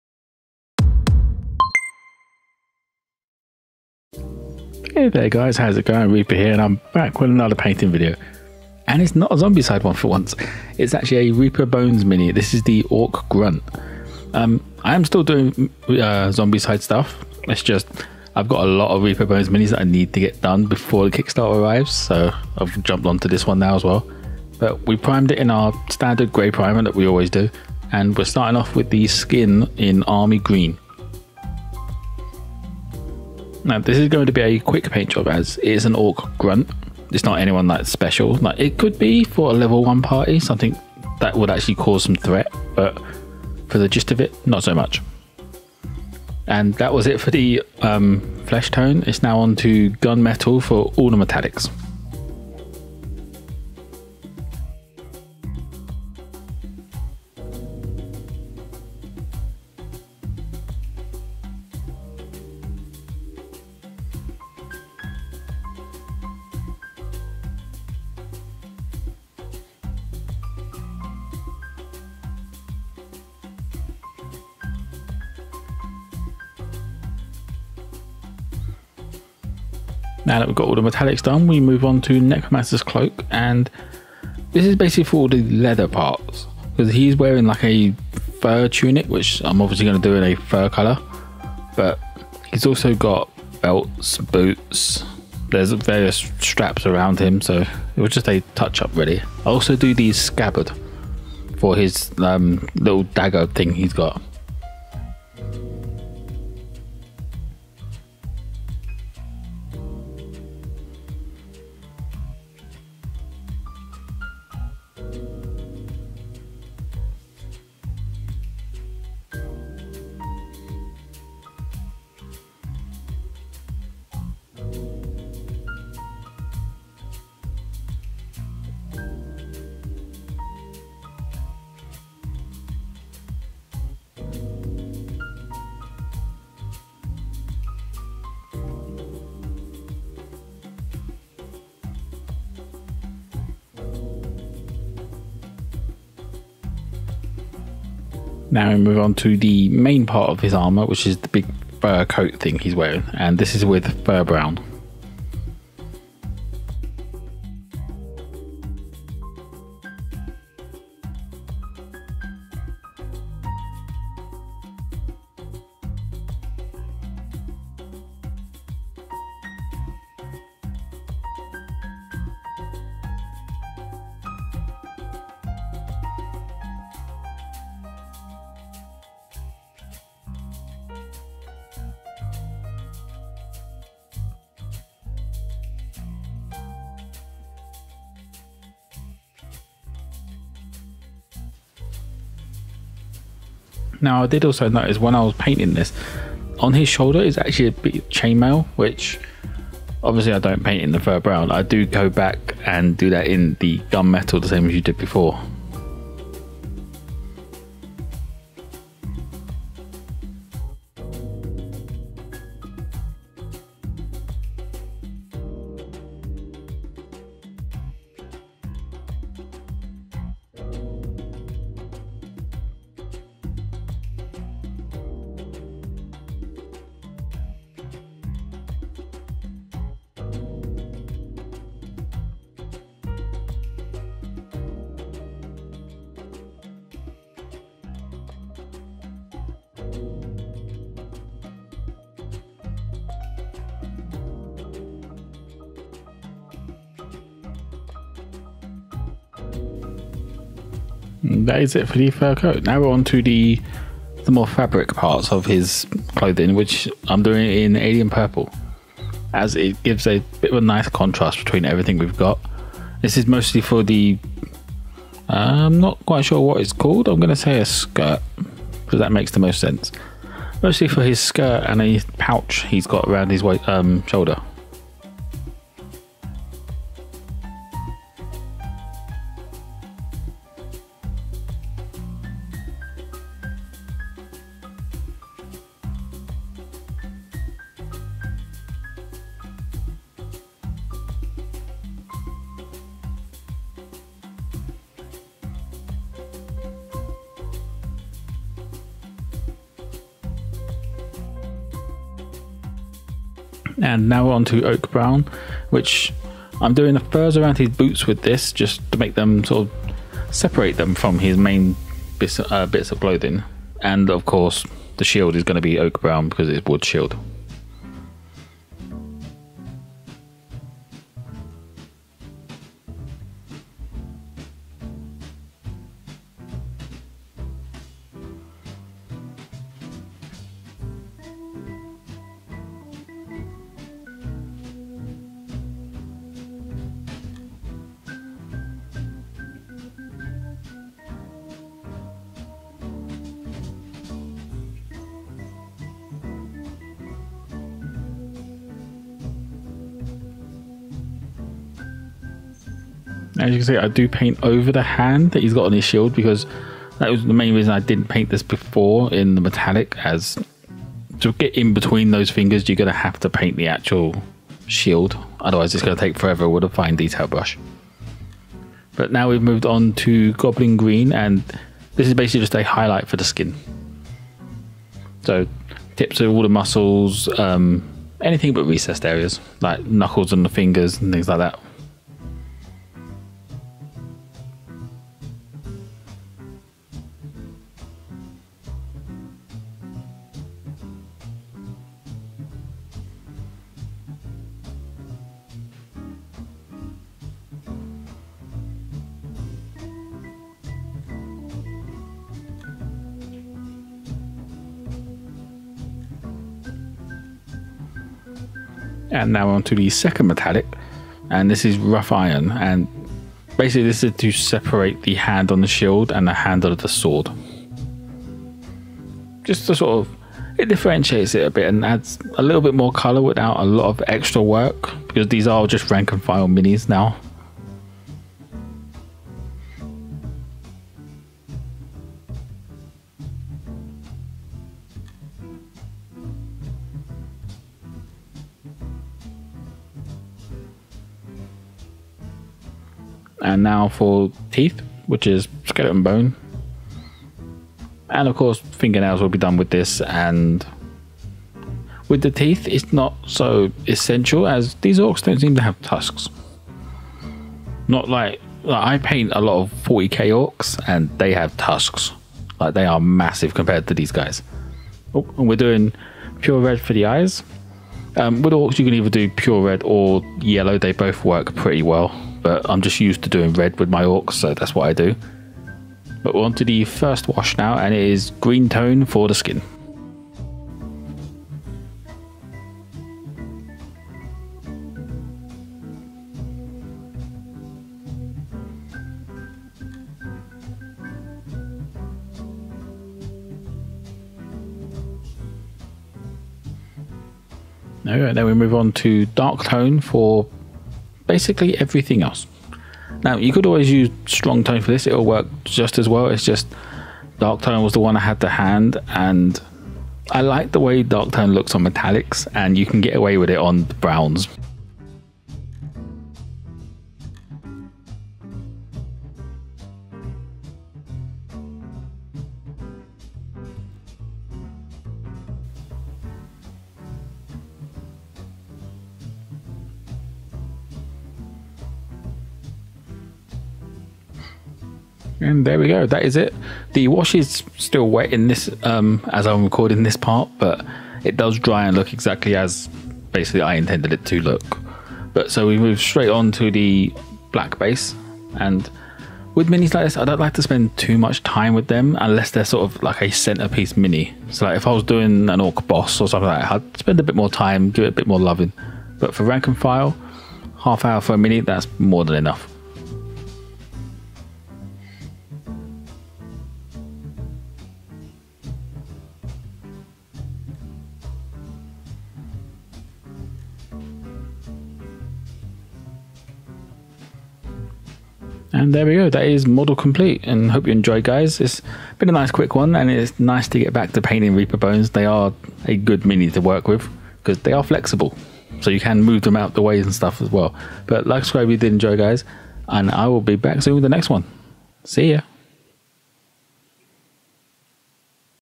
Painting Reaper Bones - Orc Grunt
I paint up a Reaper bones Orc grunt miniature.